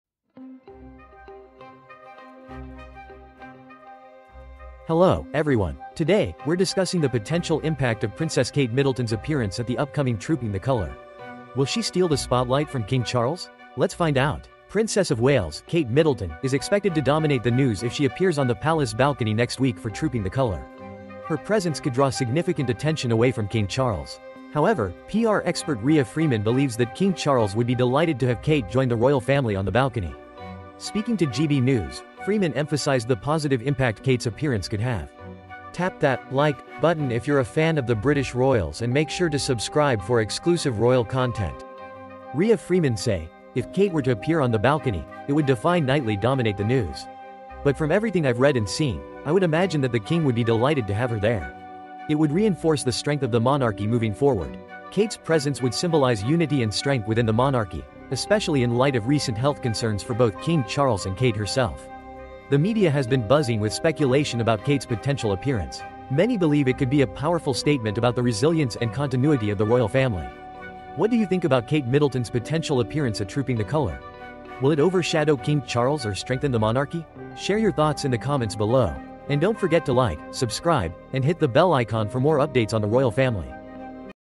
ROYALS IN SHOCK! KING CHARLES MAKES A BIG SACRIFICE FOR PRINCESS CATHERINE'S RETURN
Hello, everyone! Today, we’re discussing the potential impact of Princess Kate Middleton's appearance at the upcoming Trooping the Colour. Will she steal the spotlight from King Charles? Let's find out!

Princess of Wales, Kate Middleton, is expected to dominate the news if she appears on the palace balcony next week for Trooping the Colour. Her presence could draw significant attention away from King Charles.

However, PR expert Rhea Freeman believes that King Charles would be delighted to have Kate join the Royal Family on the balcony. Speaking to GB News, Freeman emphasized the positive impact Kate's appearance could have.

Rhea Freeman say "If Kate were to appear on the balcony, it would definitely dominate the news. But from everything I’ve read and seen, I would imagine that the King would be delighted to have her there. It would reinforce the strength of the monarchy moving forward.

Kate's presence would symbolize unity and strength within the monarchy, especially in light of recent health concerns for both King Charles and Kate herself.

The media has been buzzing with speculation about Kate's potential appearance. Many believe it could be a powerful statement about the resilience and continuity of the Royal Family.

What do you think about Kate Middleton's potential appearance at Trooping the Colour? Will it overshadow King Charles or strengthen the monarchy?